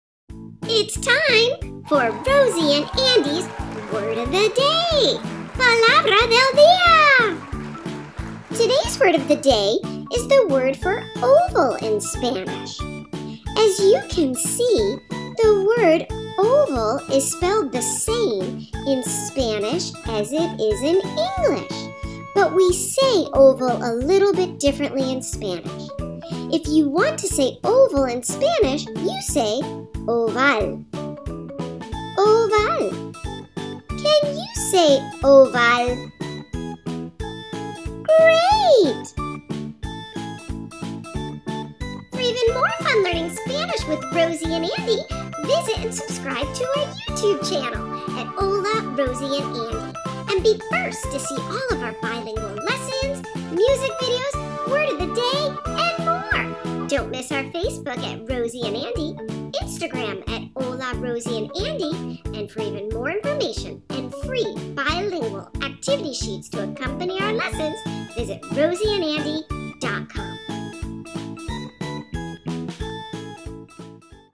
Rosie & Andy: Spanish for Kids - Word of the Day - oval / oval
Rosie & Andy share shape words in Spanish.
A bilingual lesson and free activity sheets will be available as of
Saturday, 1/23. Start having fun learning Spanish today. with Rosie & Andy!